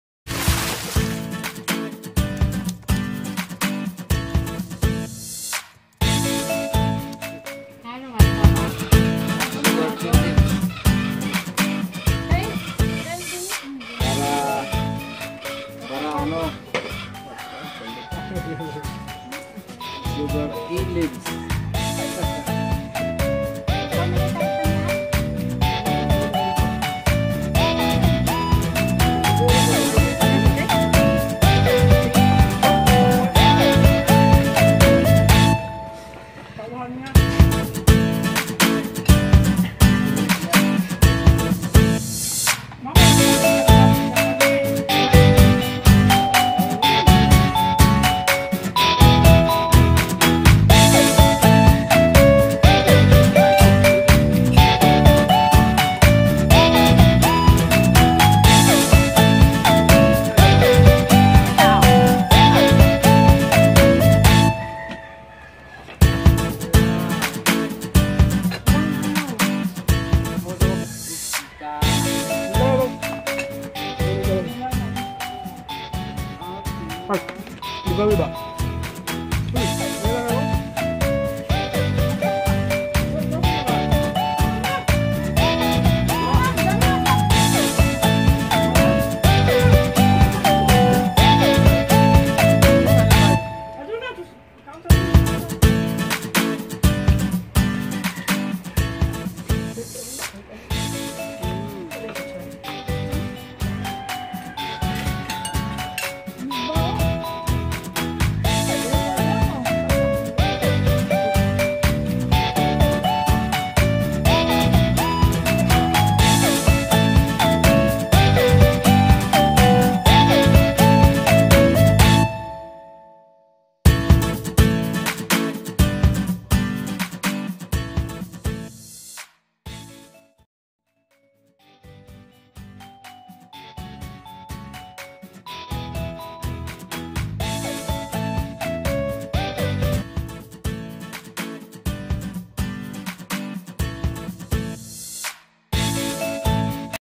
BUHAY PROBINSIYA - Philippines vacation
Bonding time with my family, we eat together outside, the life in the province (BUHAY PROBINSIYA).